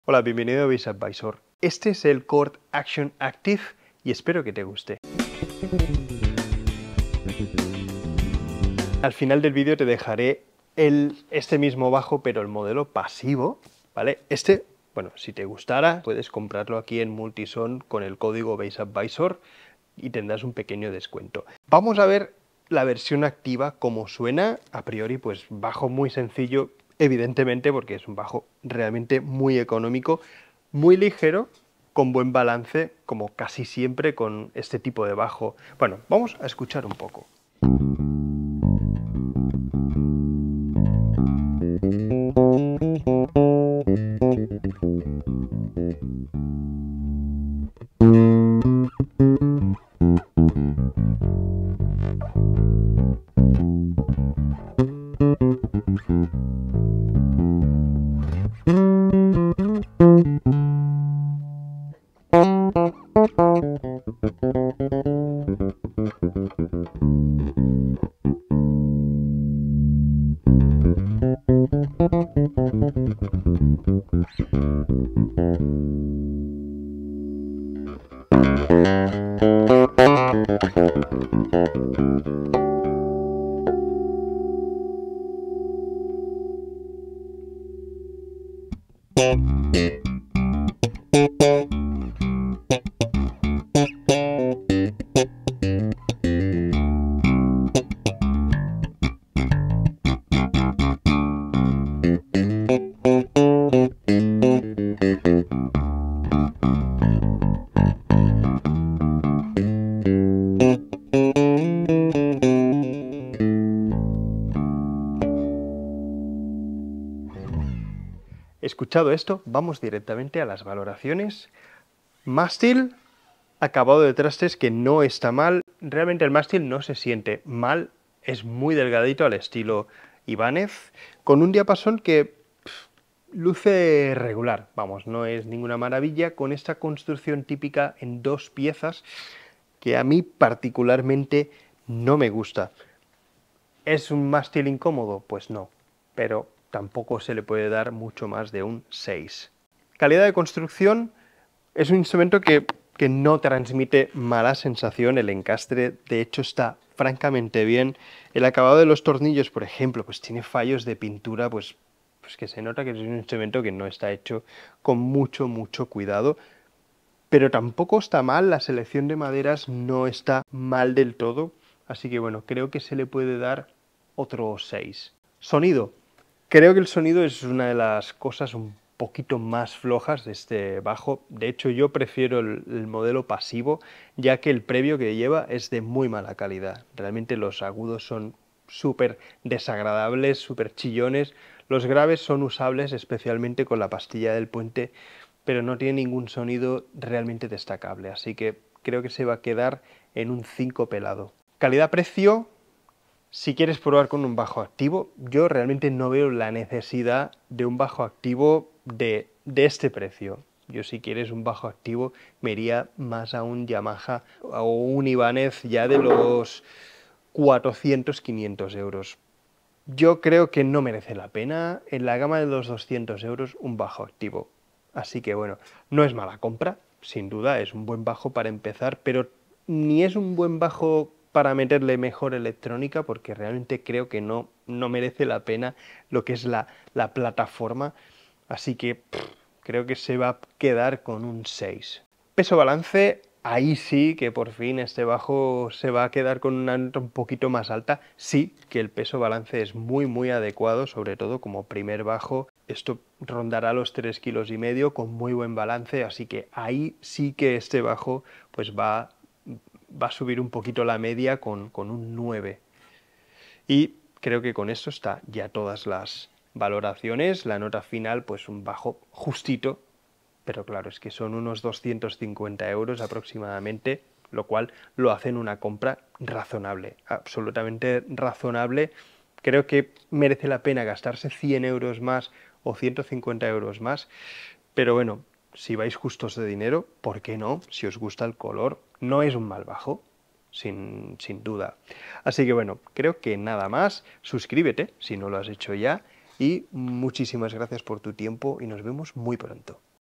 Cort Action Bass Plus. BARATO Y ACTIVO.
Aquí os traigo este análisis de este Cort Action PJ, pero en este caso el modelo activo.
¿Qué tal se defiende por el precio? Lo veremos en este vídeo.